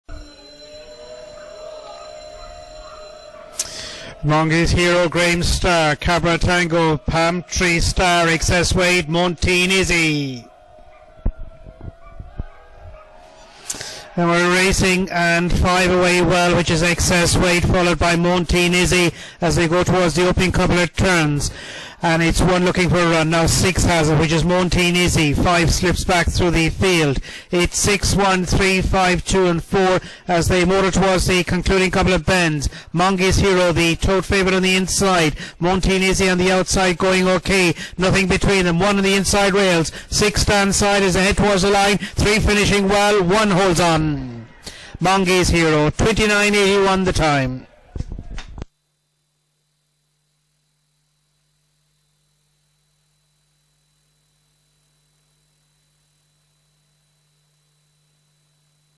IGB - Greyhound & Petworld Golden Muzzle A1 55 Round 1 Heat 2 18/05/2019 Race 11 - Limerick
Race: Greyhound & Petworld Golden Muzzle A1 55 Round 1 Heat 2
Track Name: Limerick
Date: 18/05/2019
Hour: 22:09

Results

1st    1     MONGYS HERO         5/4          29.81
2nd    6     MOANTEEN IZZY       11/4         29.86
3rd    3     CABRA TANGO         5/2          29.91
4th    2     GRAHAMS STAR        3/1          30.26
5th    5     EXCESS WEIGHT       6/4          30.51
6th    4     PALMTREE STAR       5/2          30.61

|LMK_2019_05_18_11